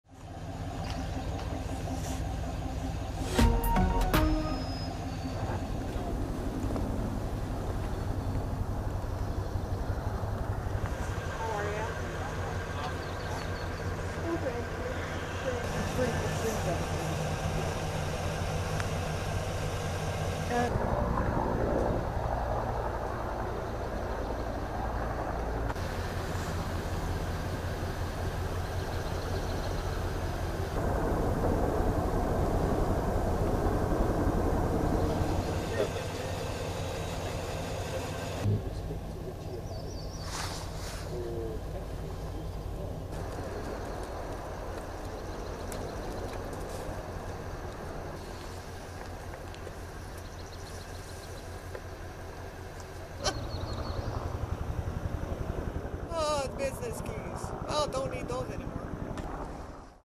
Aftermath of wildfire devastation in Western Canada
Aftermath and damage caused by the Western Canadian wildfires in the small town of Entwisle, about 100 km west of Edmonton, Alberta. Some residents have been able to return to their homes, but several fires remain out of control.